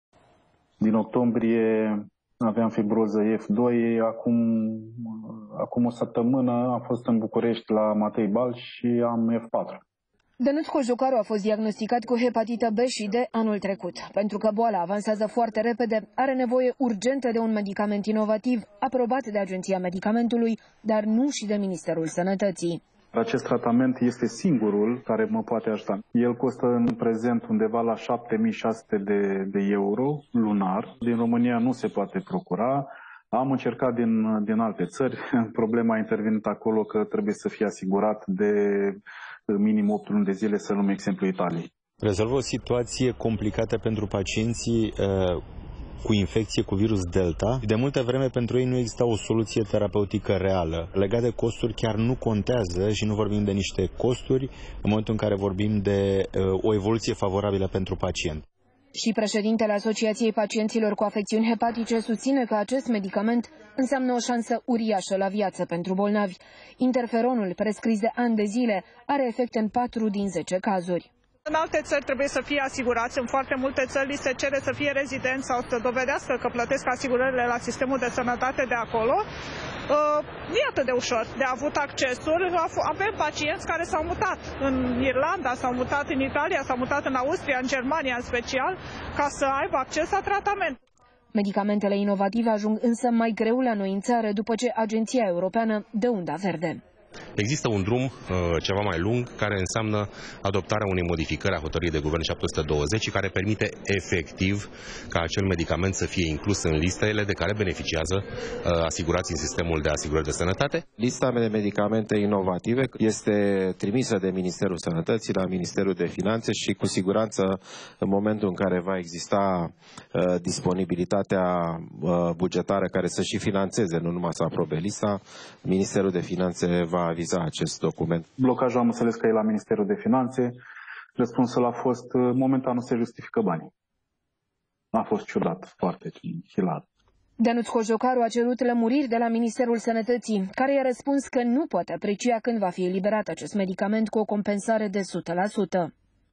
Pacienții cu hepatită D, fără tratament din cauza birocrației
Un medicament ce poate vindeca și chiar salva viața pacienților cu Hepatită B și Delta nu poate fi prescris fără aprobarea ministerului de Finanţe. E un tratament inovativ și costul total va fi suportat de stat. Zeci de alte substanțe inovative, aprobate de Agenția Europeană a Medicamentului așteaptă răspunsul final de la ministerul de Finanțe, de câteva luni bune.